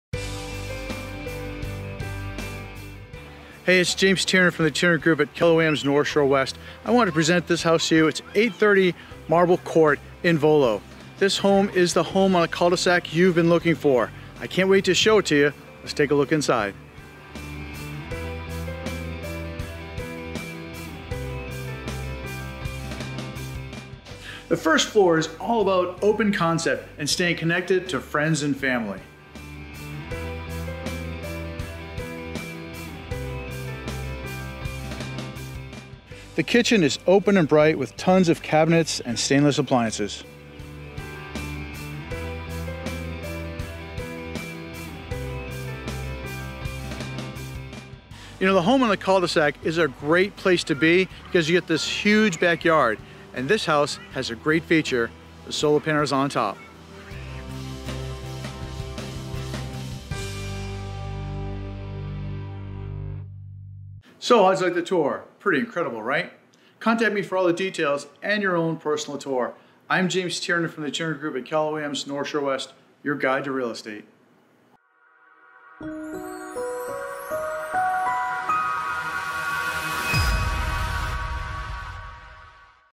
830 Marble Court - SOLD!
Welcome Home!  Beautiful cul-de-sac location, with amazing curb appeal.  Quintessential starter home for people looking for something special.  Solar Panels eliminate a high monthly ComEd bill!  Step into the Living Room and fall in love with the wide-open design.  Enjoy recessed lighting, luxury vinyl planking, and stylish ceramic tile on the first floor.  Natural light fills the space!  Enjoy cooking amazing meals in your new Kitchen, all with tons of space and featuring stainless appliances and a large pantry.  Take the party outside to the huge yard with expanded concrete patio, plenty of space for playing bags, frisbee and ball while grilling burgers and dogs. Primary bedroom suite features a spa-like bath and large walk-in closet. Unfinished basement provides ample storage and is your blank canvas to expand your liveable space with a playroom, man cave, or home gym. Close to Woodmans, and enjoy shopping in both Lake and McHenry, along with all the trails for hiking and biking.